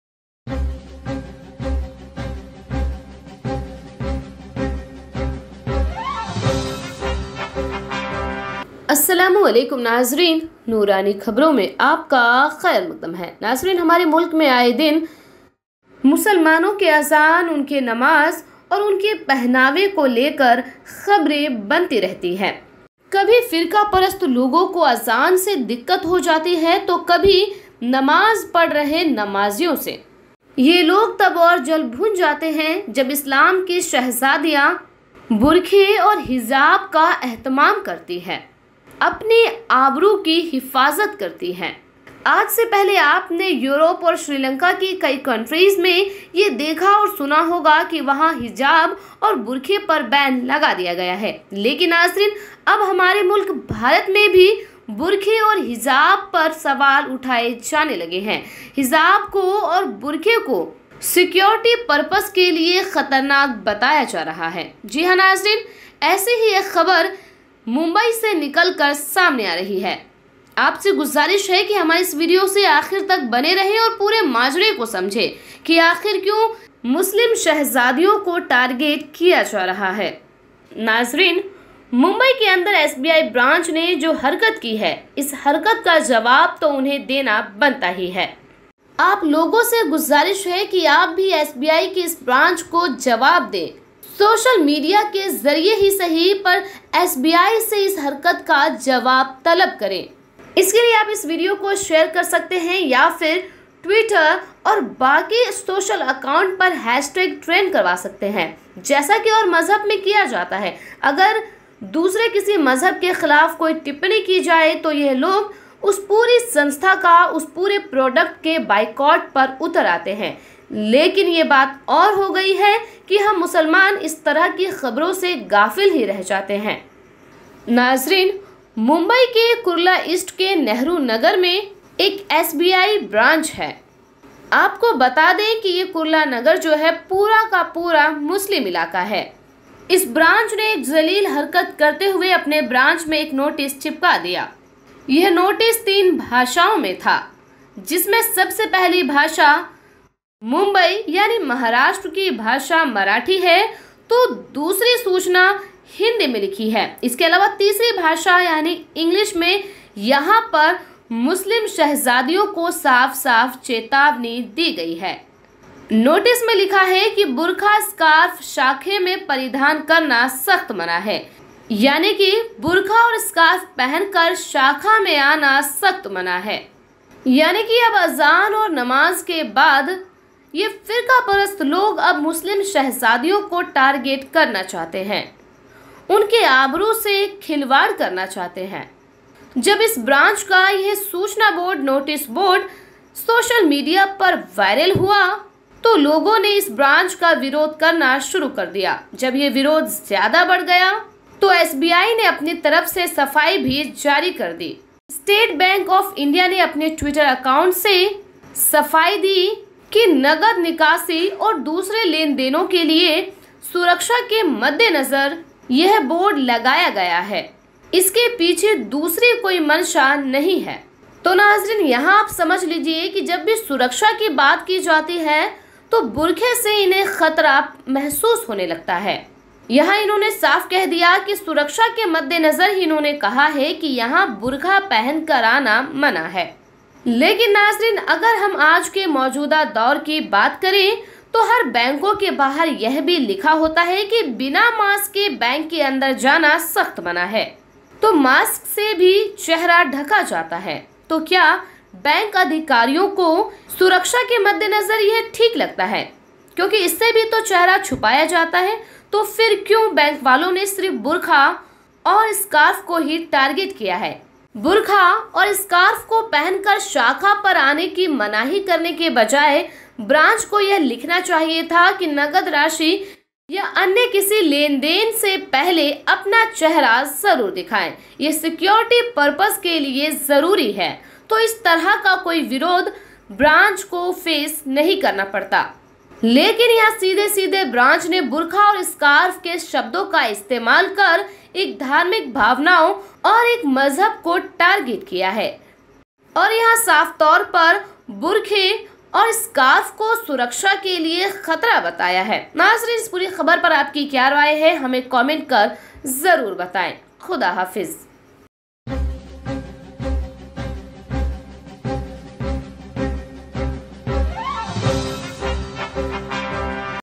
अजान और नमाज़ के बाद अब बैंक में बुर्का और हिजाब पहनकर जाने पर लगी पाबंदी, विरोध के बाद दी सफाई
About using this Content:
Video Content
Field Reporting